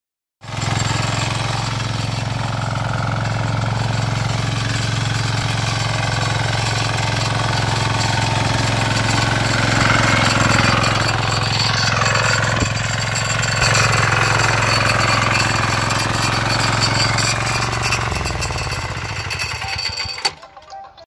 लेबर की करो छुट्टी 👌 घंटों का काम मिनटों मेंLive Demo Full power Testing Xtreme power Tiller (1)
अधिक जानकारी के लिए आप संपर्क कर सकते हैं एक्सट्रीम पावर टिलर के मालिक से

मेने
sprayman आदि कंपनियों की वीडियो बनाई हैं सभी आप देख कर ले अच्छा कौन सा है